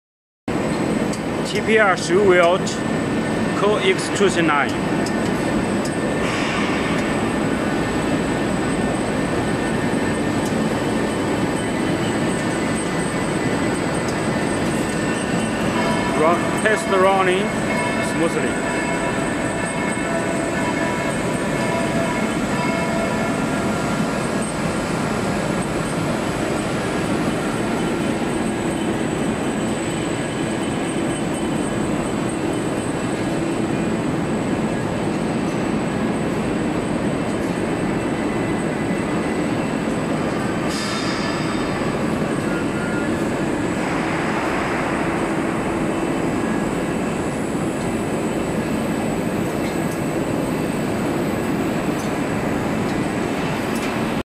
20200926 shoe welt machine
20 years professional manufacturer of the TPR shoe welt making machine , supply quality machine and turn-key service